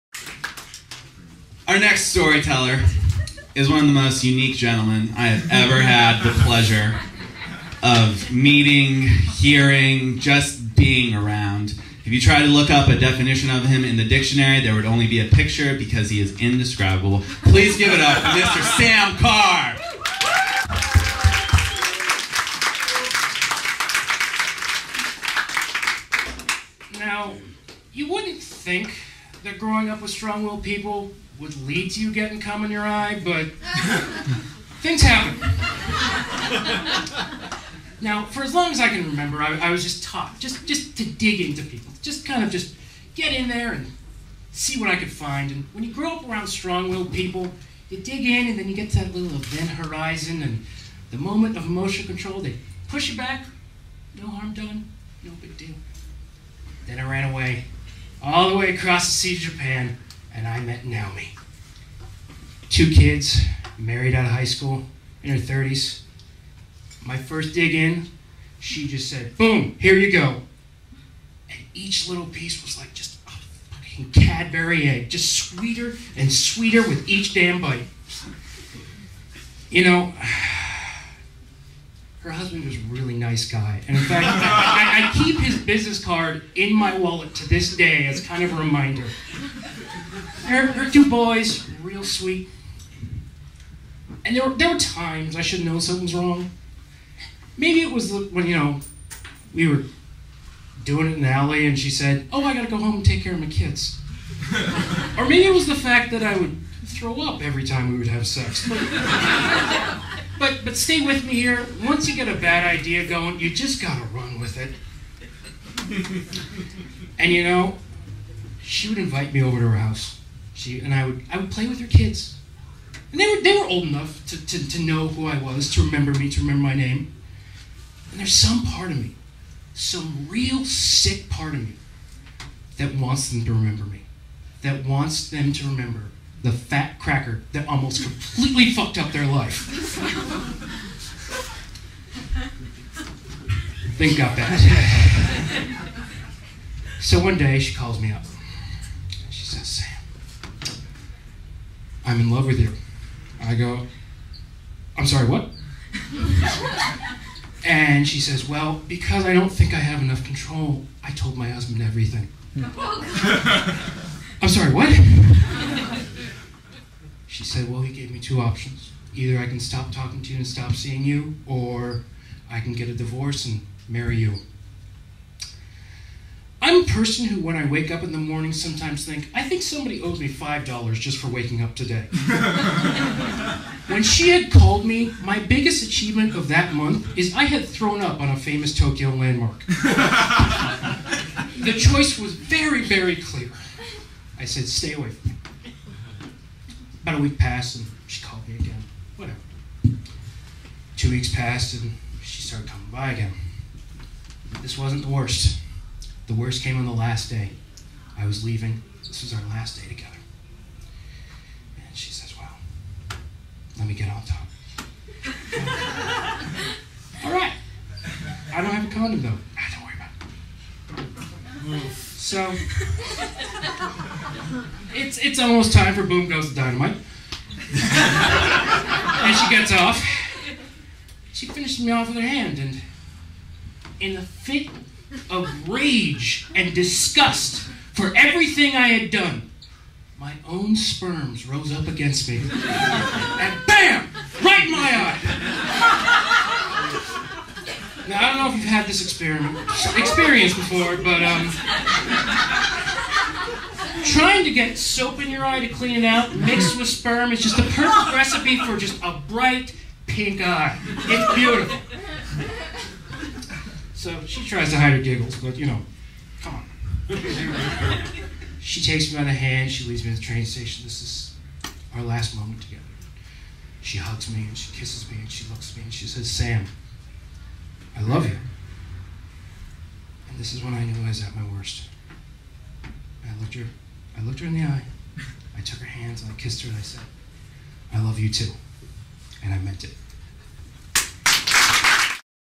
Sam Carr - Dating Married
Performances from the VERY FIRST So Say We All storytelling showcase in February 2009, "Love Is For Suckers." Filmed at Twiggs Cafe on Park in San Diego.